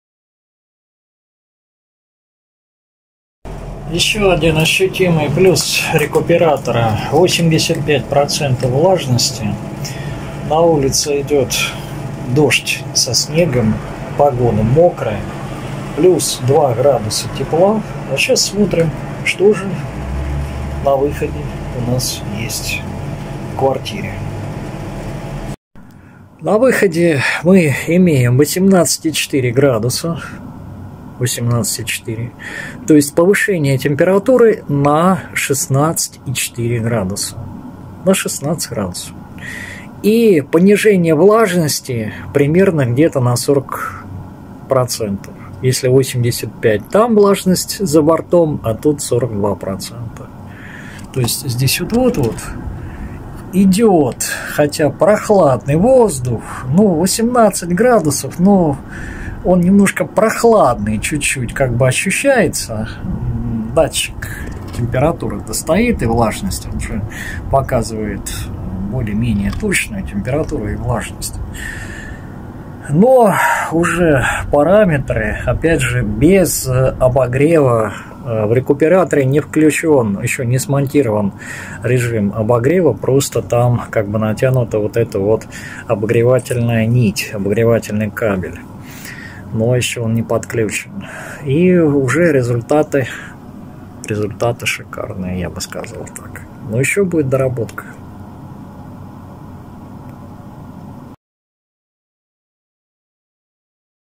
Рекуператор Микулина. Часть 5. И всё же, он работает! КБ Микулина. МикЛаб. (14.11.2024)
Первое испытание Рекуператора Микулина
Сравнение температуры и влажности на улице и в комнате.
Простой Рекуператор своими руками.
Рекуператор конструкции А.Л.Микулина.
Система УМНЫЙ ДОМ.

КБ Микулина - Проект МикЛаба
MIKLAB - ROBOTICS
РОБОТОТЕХНИКА МИКЛАБ

МикЛаб - Лаборатория Андрея Микулина